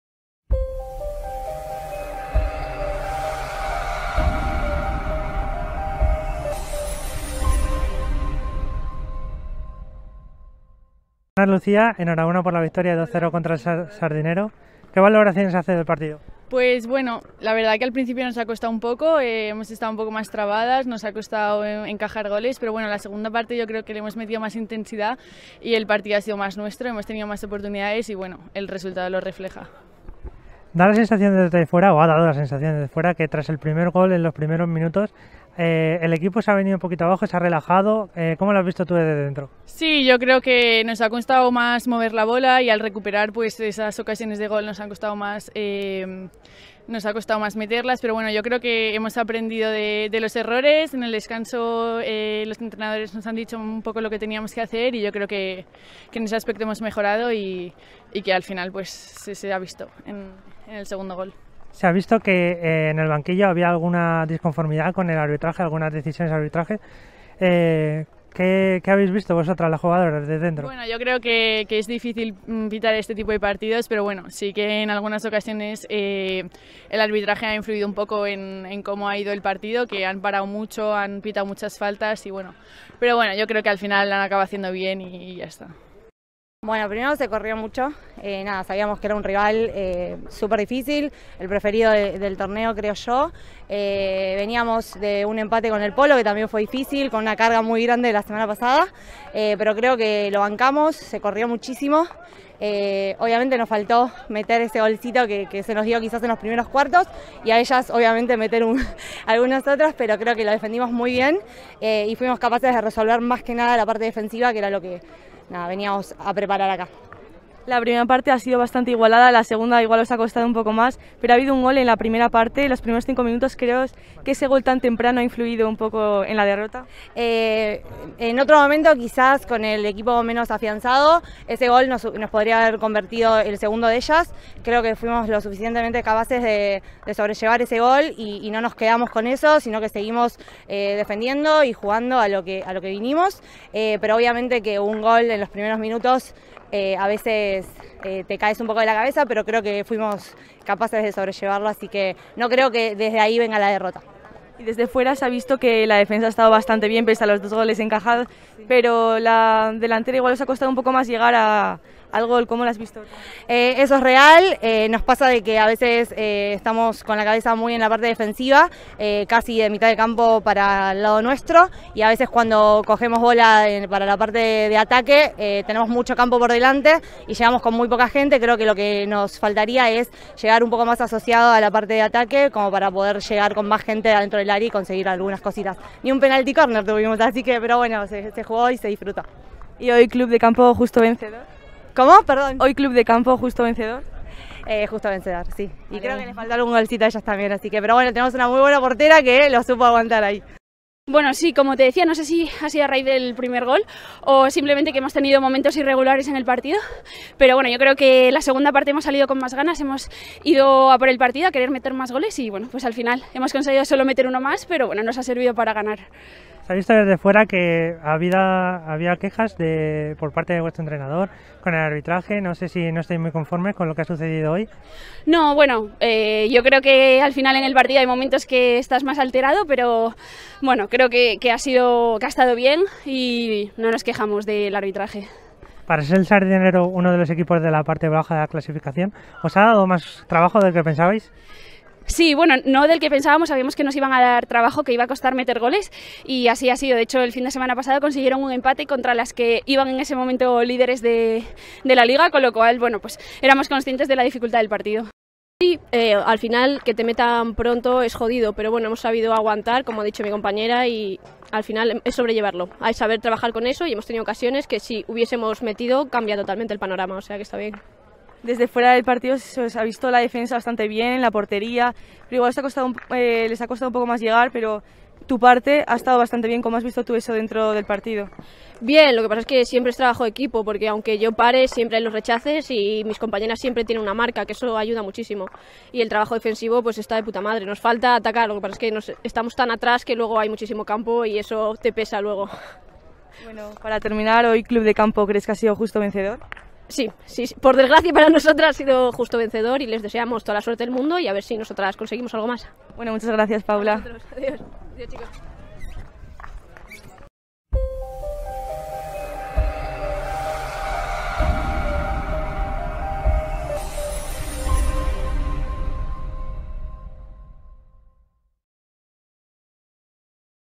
Club de Campo -- Sardinero HC (Hockey femenino) 20-03-2022
Entrevistas con Lucía Abajo, Laureana De Felice, María López y Paula Ruiz tras el Club de Campo -- Sardinero HC (hockey femenino) para